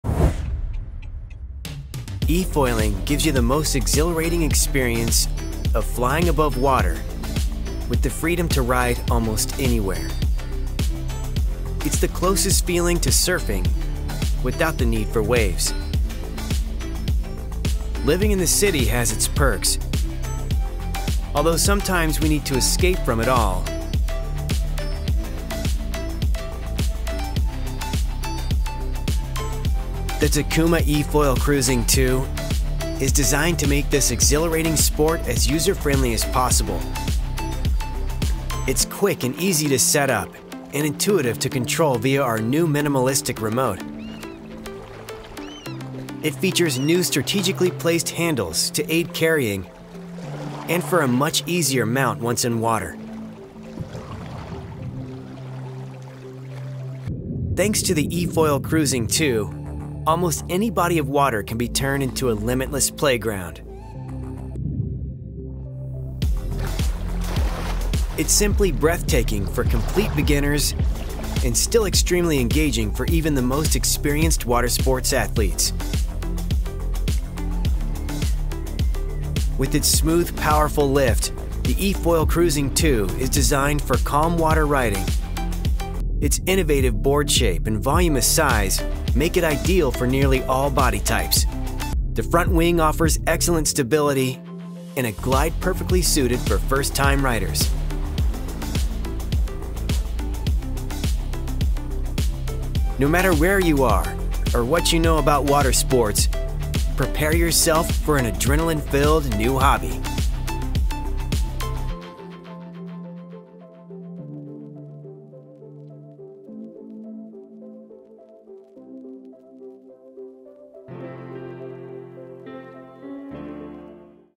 Takuma Cruising 2 eFoil
The all new Takuma Cruising 2 Efoil is perfect for beginners starting out their journey and experts to hone their skills. The Cruising 2 glides above the water with ease!